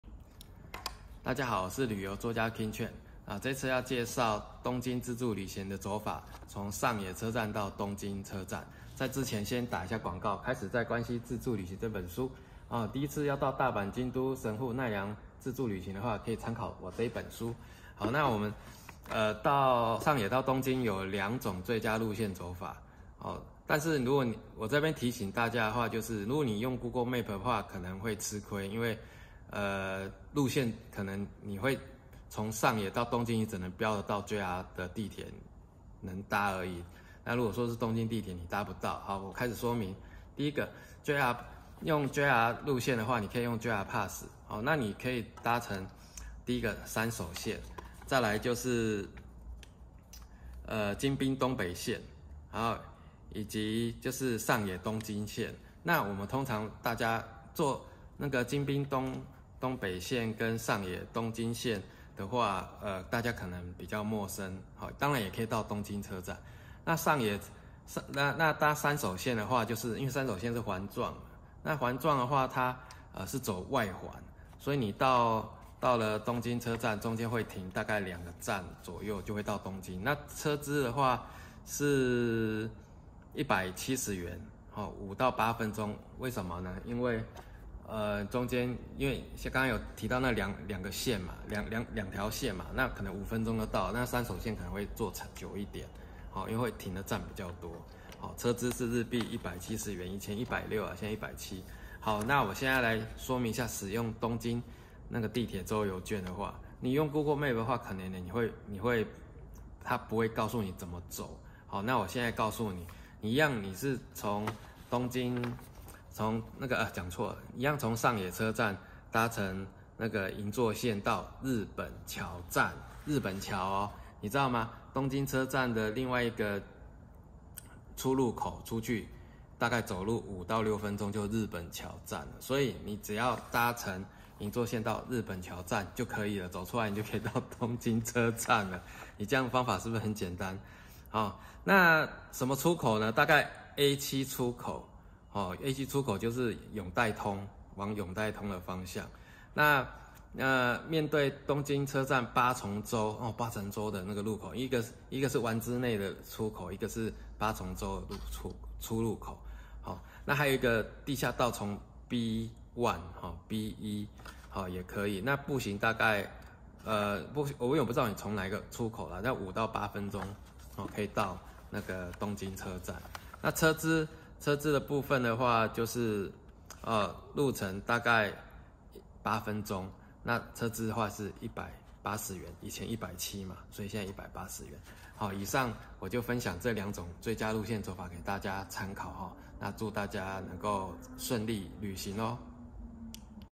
從東京上野駅到東京駅搭乘地鐵最佳的路線走法，相信google map?還是相信經驗走法？King Chen旅遊分享
從上野車站到東京車站搭乘地鐵有兩種最佳走法，這要看你身上是使用哪種周遊券，本篇有介紹使用JR PASS及東京地鐵周遊券的搭乘方式，供讀者們參考！